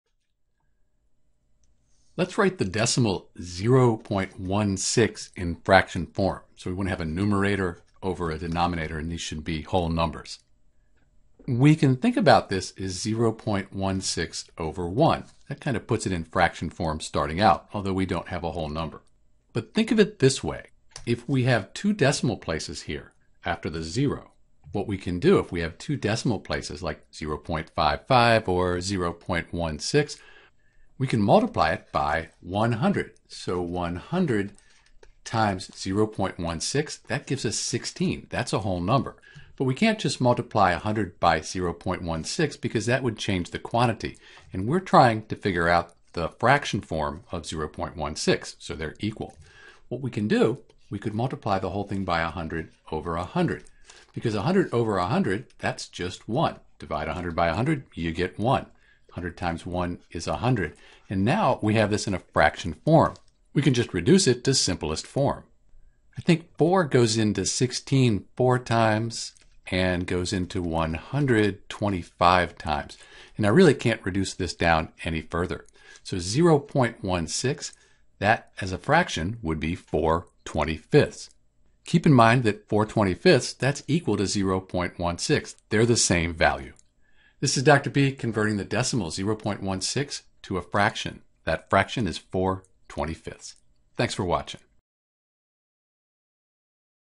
0.16 as a Fraction (simplified form)
In order to write 0.16 in fraction form, or any decimal as a fraction, we need to first get it to a whole number over a whole number.  In this case if we consider 0.16 to be 0.16/1 we can multiple both the numerator and the denominator by 100:

100/100   *  0.16/1

When we express numbers as fractions we need to make sure we have both the numerator and denominator in their simplest form.  We can do this by dividing both by a common factor.